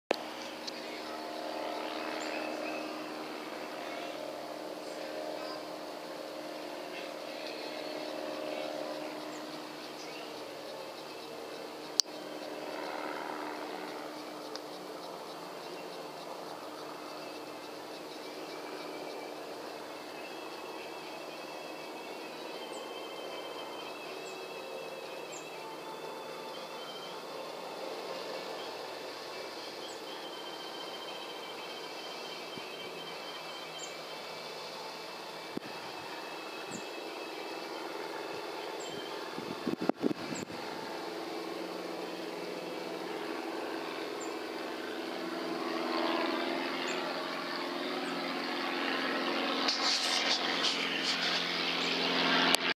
Two More Rats!
My target was the smaller of the 2, but he never gave me a real clear shot.  He will be back another day!